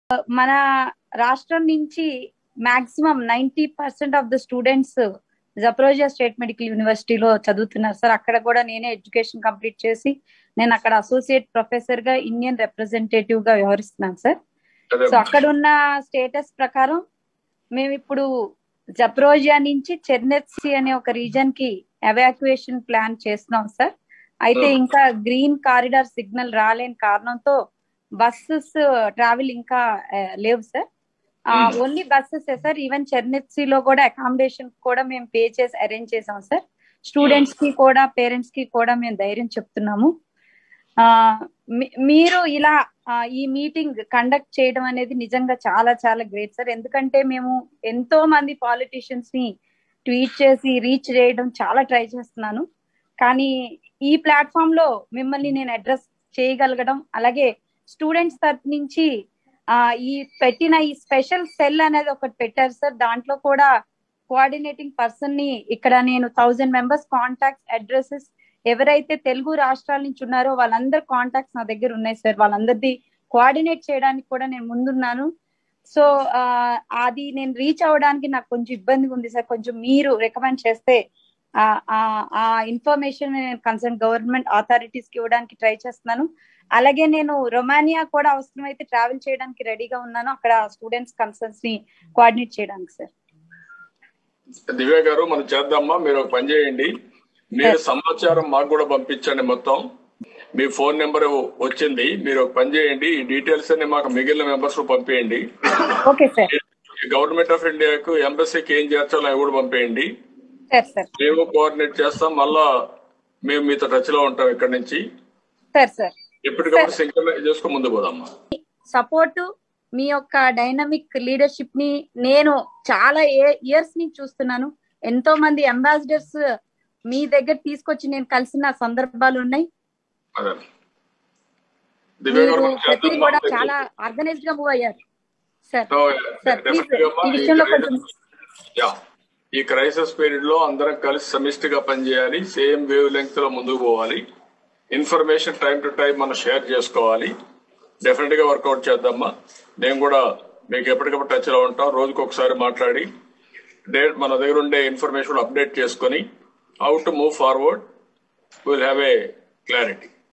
ఉక్రెయిన్ తెలుగు ప్రజలతో ఫోన్ కాల్ మాట్లాడిన చంద్రబాబు | Chandrababu | OPEN TALK
Category
News & Politics